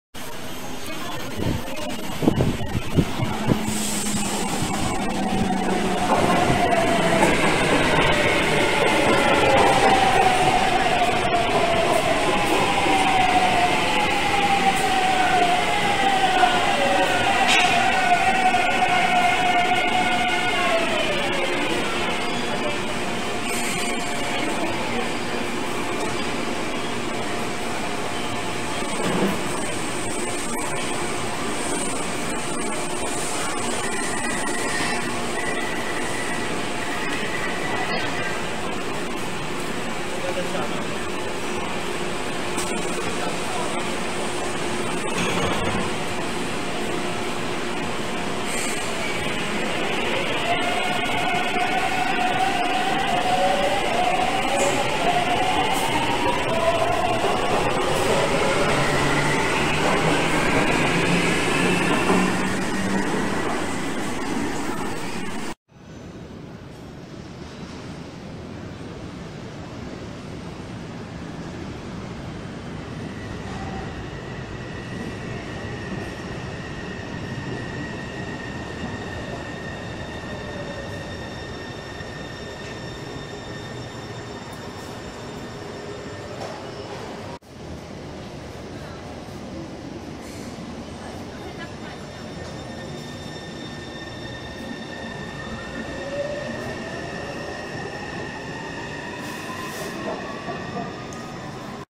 【捷運列車紀錄EP.7】臺北捷運C301型電聯車043/044編組GTO-VVVF與更換IGBT-VVVF後進出站之比較 | Taipei MRT | 粘粘的鐵道攝影
臺北捷運C301型電聯車043/044編組

2015/8/16臺北捷運C301型電聯車芝山站進站+出站
2015年時C301型部分編組仍採用美國西屋GTO-VVVF推進系統

2020/10/31臺北捷運C301型電聯車北投站進站+雙連站出站
2020年時C301型之推進系統已全數汰換為加拿大龐巴迪IGBT-VVVF推進系統